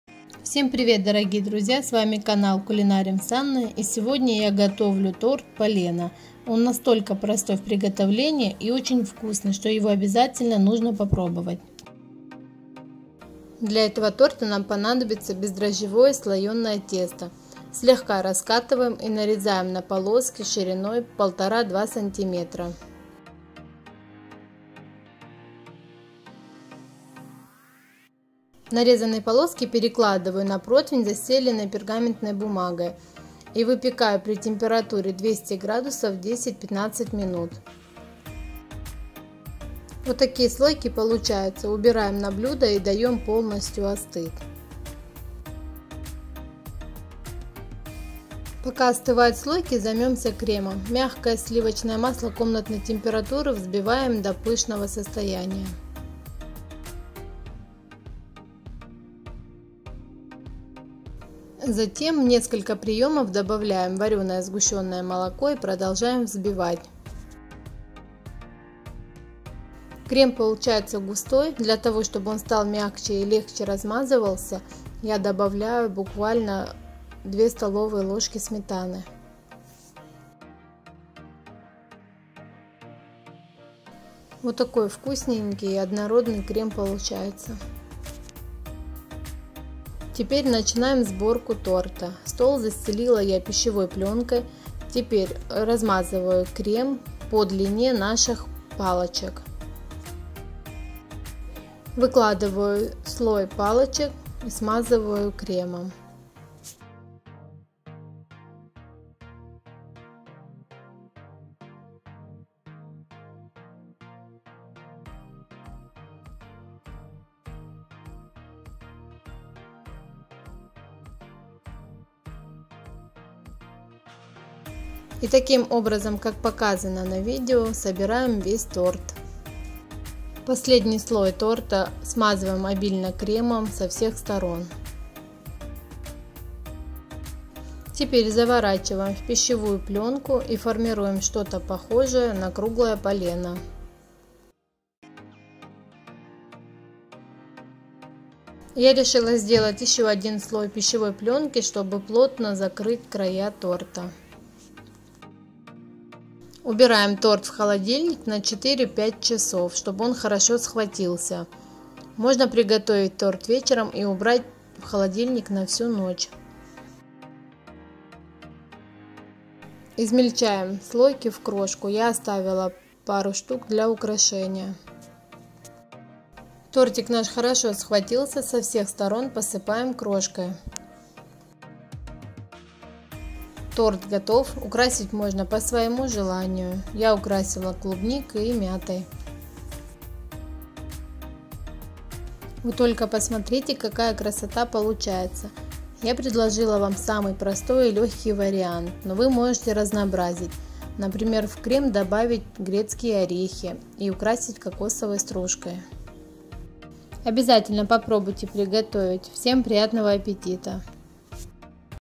Торт ПОЛЕНОс варёной сгущенкой/всего три ингредиента
Список ингредиентов :
☑️Бездрожжевое слоёное тесто-500гр
☑️Сливочное масло-230гр
☑️Вареное сгущеное молоко-1б(380г)